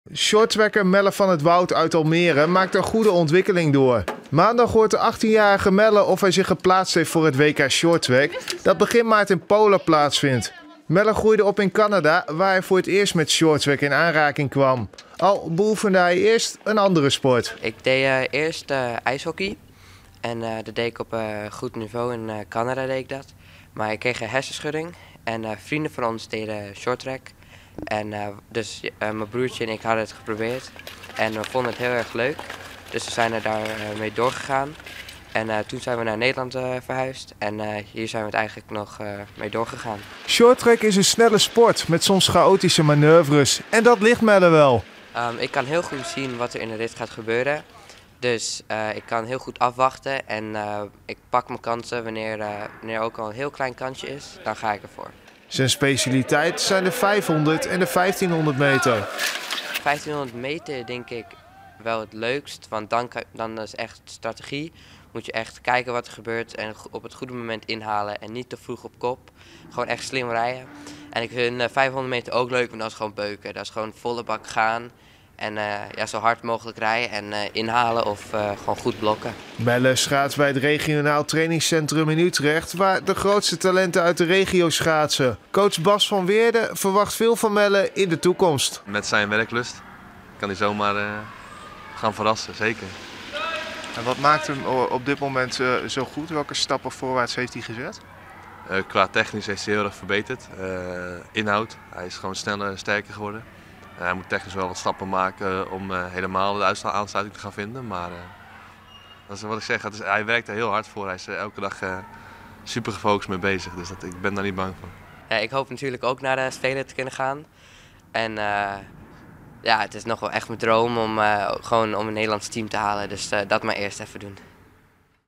Shorttracker Van 't Wout droomt van Spelen
Programma: Sport
Verslaggever: Jeroen Kuiper
Locatie: Almere

De 18-jarige Almeerse shorttracker Melle van 't Wout droomt van deelname aan de Olympische Spelen in Beijing van 2022. Maandag hoort de Almeerder of hij mag deelnemen aan het WK voor junioren in Polen. Dit toernooi vindt van 2 tot en met 4 maart plaats.

Van 't Wout traint bij het regionaal trainingscentrum in Utrecht, samen met zijn één jaar jongere broertje Jelle. Volgens coach Bas van Weerden heeft Van 't Wout potentie. De specialiteit van de 18-jarige ligt op de 500 en 1500 meter.

Van 't Wout groeide op in Canada, waar hij begon met ijshockey. Door een hersenschudding stopte hij met die sport en ging hij verder met shorttrack.